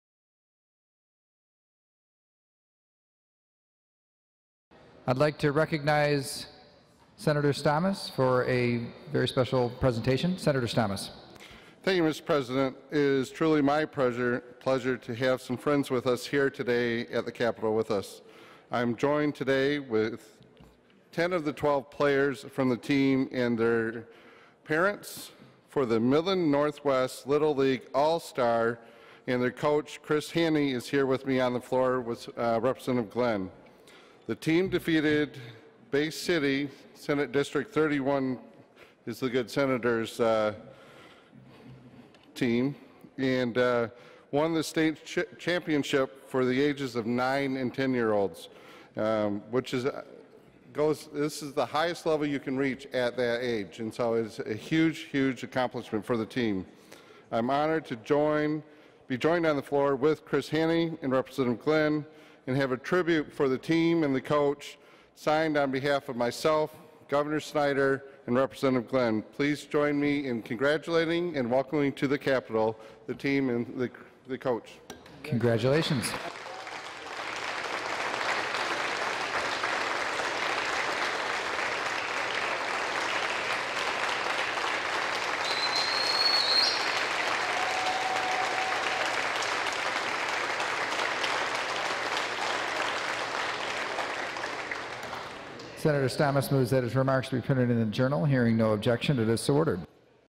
Sen. Jim Stamas recognizes the Midland Northwest Little League All Stars as Stat Champs
Senator Jim Stamas R- Midland: The Midland Northwest Little League All Stars were recognized by Senator Jim Stamas and the Michigan Senate, the baseball team was crowned the state  champions of 9 and 10 year old division.